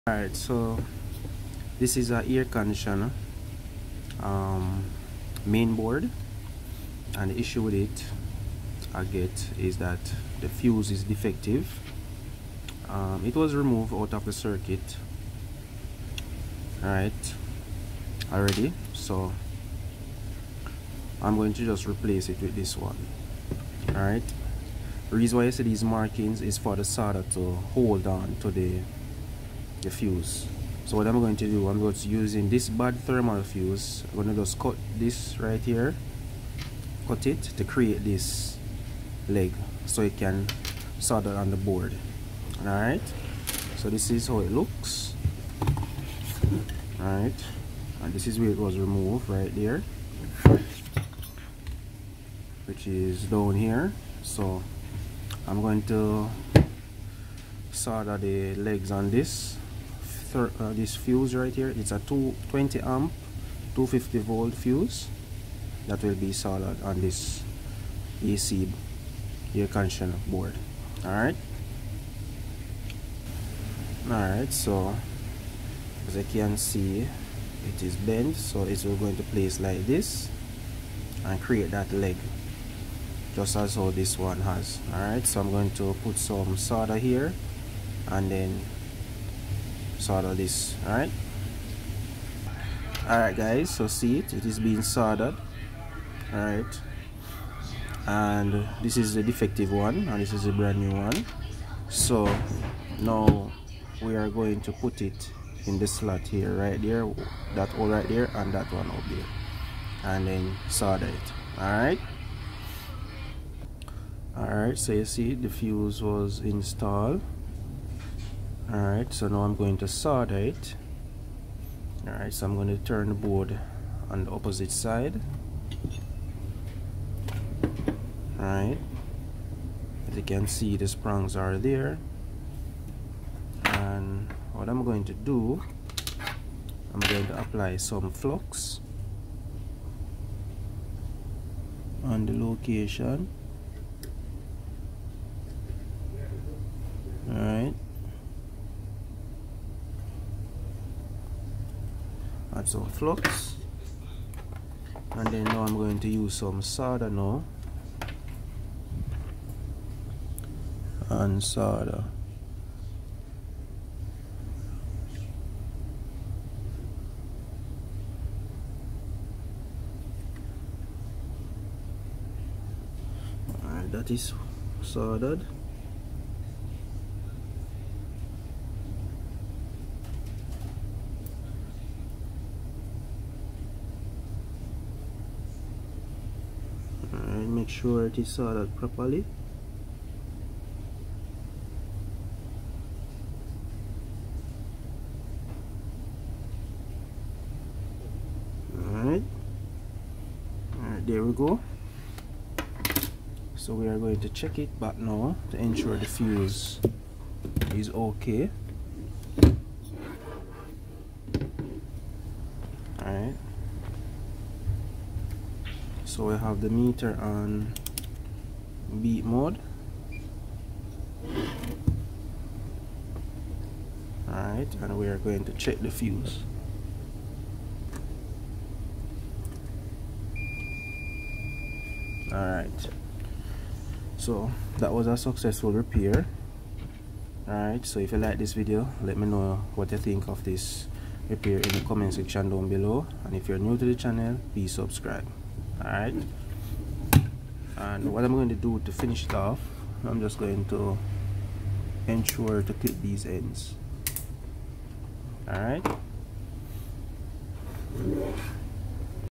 Air conditioner control board KFR-35W/BP3N1 | How to replace blown fuse.
In this video I demonstrate a air conditioner control board KFR-35W/BP3N1 that has defective fuse. The fuse was replace